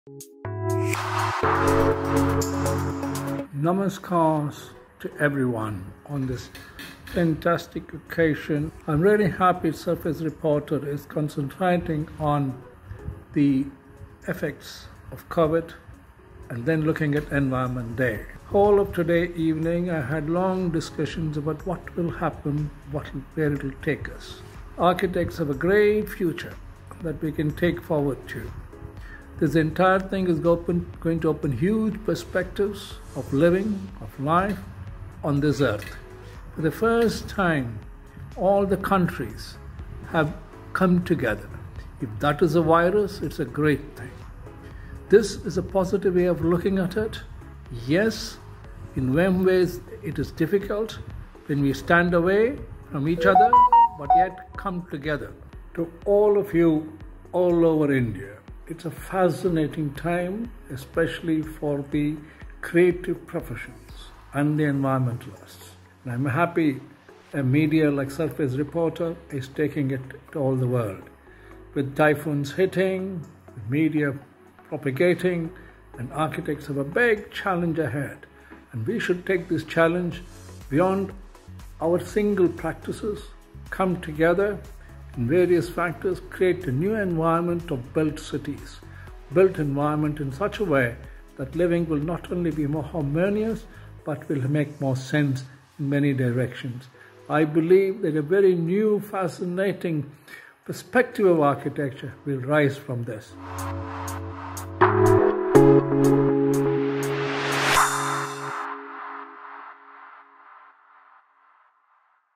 TIME FOR NATURE | Prof Krishna Rao Jaisim exclusive interview | World Environment Day | SR
Timefornature "Architects have great future that we can take forward. The Pandemic
 is going to open huge perspectives of life on this Earth. For the first time ever, all the countries have come together which is a virus, it’s a great thing. Yes, in one way it is difficult when we stand away from each other but yet come together."
Ar Prof Krishna Rao Jaisim

Ar Krishna Rao Jaisim, the former chairman of the Indian Institute of Architects, Karnataka, has been in the labyrinth of architectural creations for over 5 decades now. Although he considers his architectural journey an accidental choice, the talent can be traced back in his bloodline to his grandfather being the chief architect to the Maharaja of Mysore until the 1940s.

On the occasion of World Environment Day, Surfaces Reporter presents "Time for Nature", a series dedicated to the thoughts of Architecture Think-tank about Environment and future of Earth. Today, for the whole day, you will see Exclusive videos/ Thoughts by renowned architects.

Keep reading Surfaces Reporter